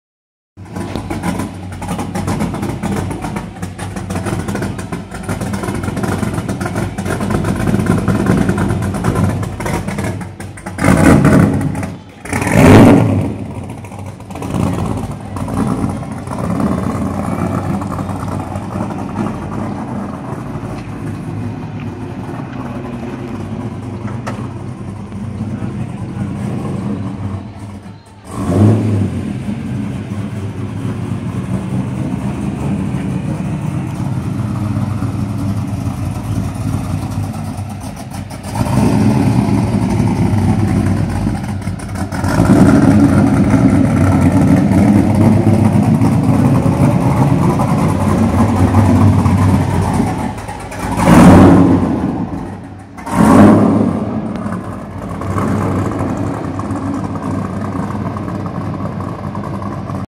Redhoonigan Idle vid
Waxed her late night and she sounded too sexy not to take a vid of her...